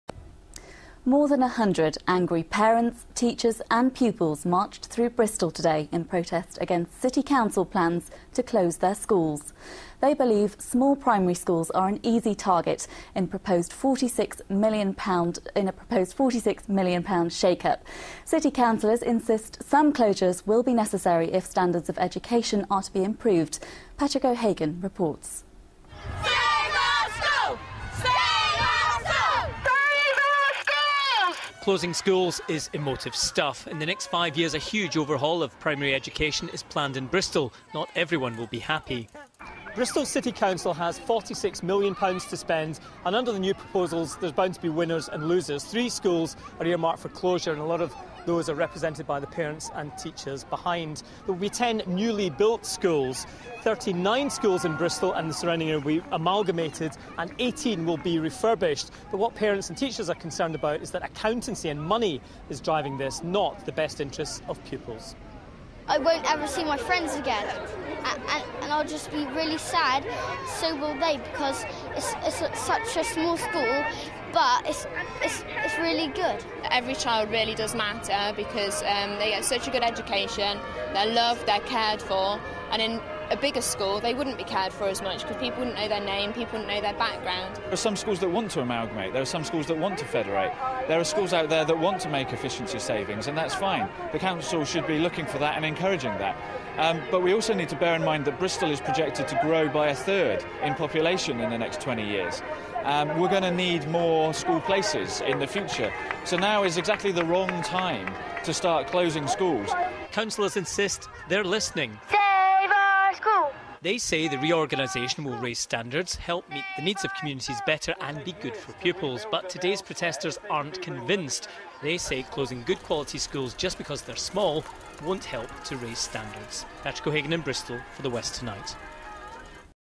School march
Local TV news footage of the anti-closure School march on 19th July